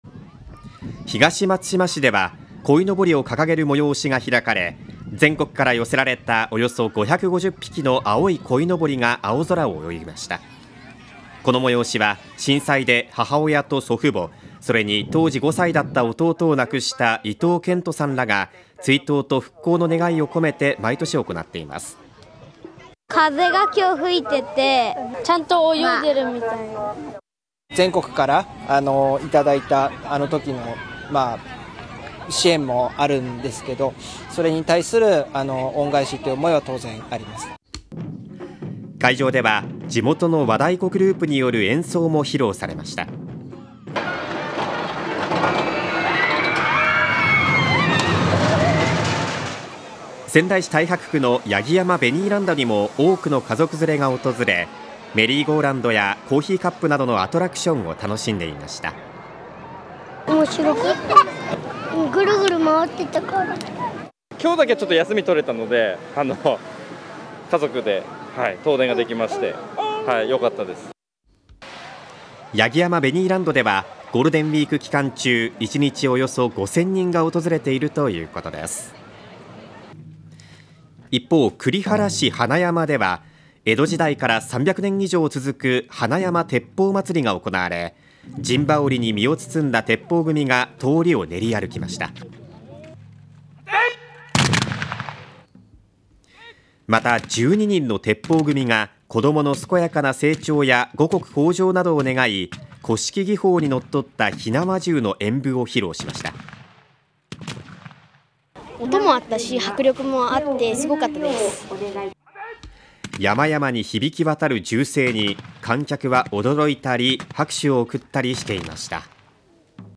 「こどもの日」 復興願い550匹の「青い鯉のぼり」掲げる 子ども健やかな成長願う伝統の「花山鉄砲まつり」 遊園地には1日約5000人が来園 行楽地やイベント会場にぎわう 宮城
こどもの日の5日、宮城県内の行楽地やイベント会場は多くの家族連れなどで賑わいました。
東松島市では鯉のぼりを掲げる催しが開かれ全国から寄せられたおよそ550匹の青い鯉のぼりが青空を泳ぎました。
この催しは、震災で母親と祖父母、それに当時5歳だった弟を亡くした伊藤健人さんらが追悼と復興の願いを込めて毎年行っています。
男の子「風も吹いていてちゃんと泳いでいるみたい」
伊藤健人さん「全国からいただいた時の支援もあるんですけれど、それに対する恩返しという思いもあります」
会場では地元の和太鼓グループによる演奏も披露されました。仙台市太白区の八木山ベニーランドにも多くの家族連れが訪れ、メリーゴーランドやコーヒーカップなどのアトラクションを楽しんでいました。
子ども「面白かった。ぐるぐる回っていたから」
訪れた人「きょうだけ休みが取れたので家族で遠出ができたのでよかった」
八木山ベニーランドではゴールデンウイーク期間中、1日でおよそ5000人が訪れているということです。
一方、栗原市花山では江戸時代から300年以上続く花山鉄砲まつりが行われ陣羽織に身を包んだ鉄砲組が通りを練り歩きました。
また、12人の鉄砲組が子どもの健やかな成長や五穀豊穣などを願い古式技法に則った火縄銃の演武を披露しました。
子ども「音も迫力もあって凄かった」
山々に響き渡る銃声に観客は驚いたり、拍手を送ったりしていました。